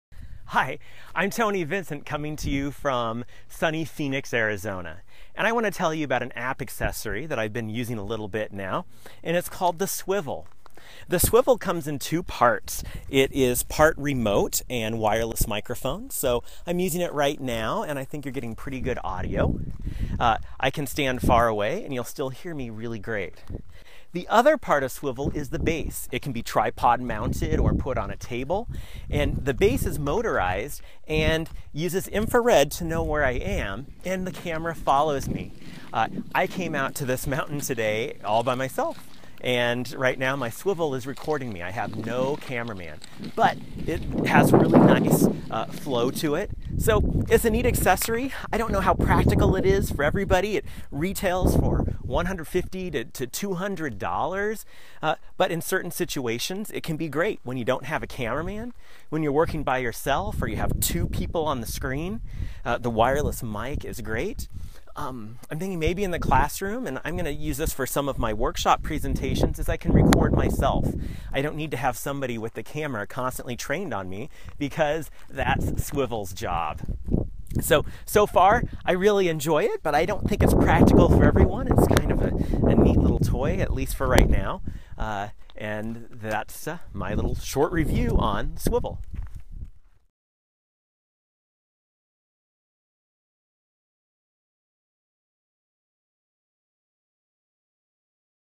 Swivl Review
Tony Vincent's short review of Swivl, "the complete personal, portable cameraman." This video was filmed by a Swivl in Phoenix, Arizona.